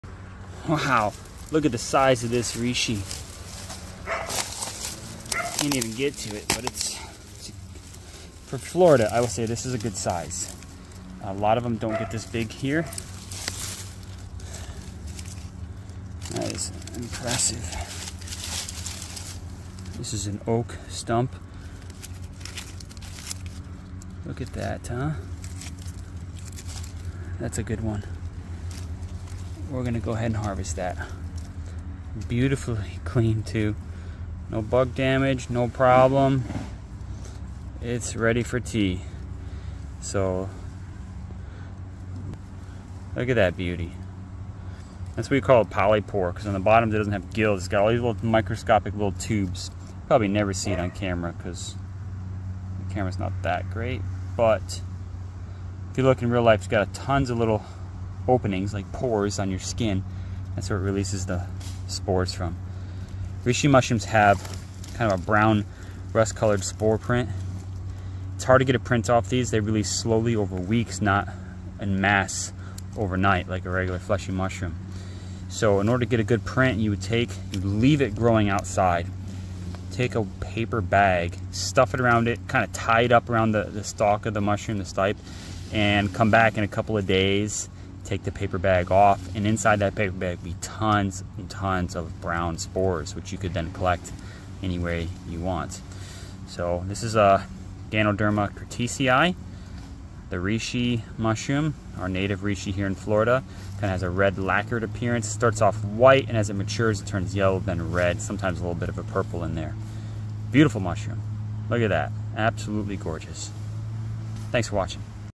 Perfect Reishi in the Church Garden
Today in the church garden I found a perfect Reishi mushroom growing on an oak stump. 🍄✨ I harvested it for tea and shared quick tips on identifying Reishi, how its spore print works, and the key features to look for (varnished cap, white underside, woody texture).

Reishi (Ganoderma) has been treasured for centuries for tea and tinctures—finding one this pristine in the garden felt like a blessing. 🌿